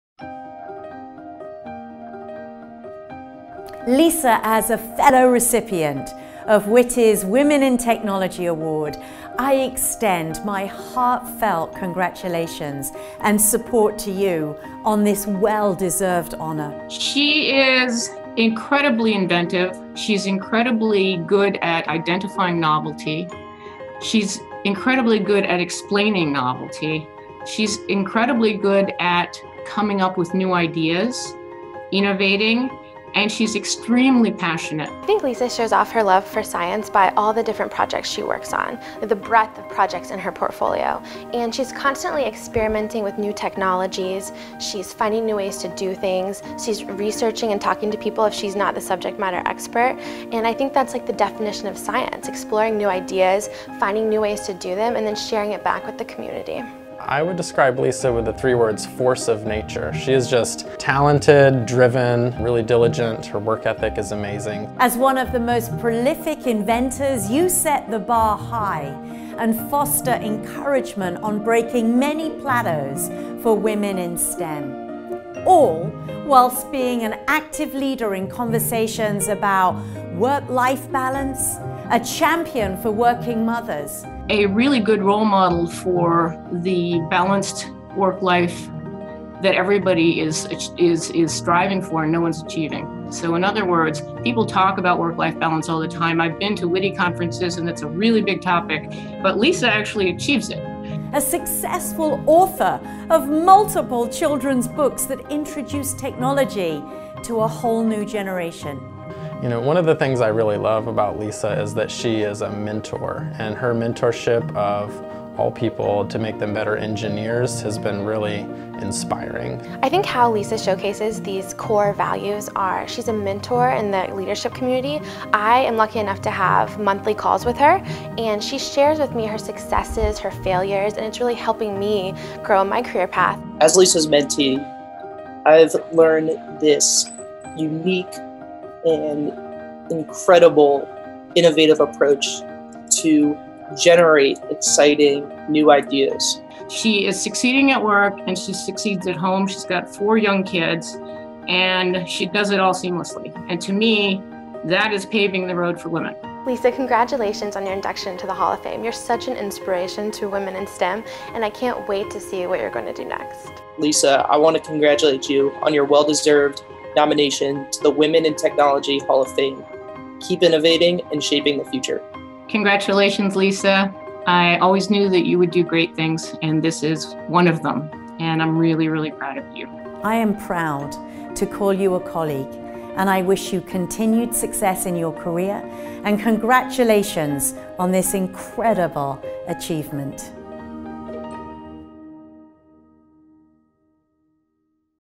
Lisa Seacat DeLuca, Technology Strategist, Cognitive Incubation Lab, IBM, 2017
Lisa Seacat DeLuca – WITI Hall of Fame Inductee

🎉 Congratulations to Lisa Seacat DeLuca, a trailblazing technologist, innovator, and mentor, on her induction into the Women in Technology International (WITI) Hall of Fame!

Lisa is a force of nature in the world of technology. As the most prolific female inventor in IBM history, with over 800 patents filed and 600+ granted, she has paved the way for innovation in AI, blockchain, IoT, cloud computing, and more. Currently serving as Vice President of Engineering & Product at Unstoppable Domains, Lisa is shaping the future of Web3 identity and decentralized technology.

Beyond her groundbreaking technical contributions, Lisa is a mentor, author, and champion for work-life balance. She has empowered countless professionals through her leadership, authored two children's books to introduce young minds to technology, and continues to inspire the next generation of STEM innovators.

🔹 Key Highlights:
✨ Award-Winning Innovator – MIT Innovators Under 35, LinkedIn Top Voices in Technology, IBM Master Inventor
✨ Technology Leader – Driving Web3 innovation at Unstoppable Domains
✨ Mentor & Advocate – Championing diversity, work-life balance, and women in STEM
✨ Author & Speaker – TEDx speaker and creator of “A Robot Story” & “The Internet of Mysterious Things”

💡 Lisa's impact reaches far beyond her personal achievements—she uplifts the entire tech community through her relentless drive to innovate, mentor, and inspire.

🎥 Watch this video to hear from those who have witnessed Lisa’s incredible journey firsthand.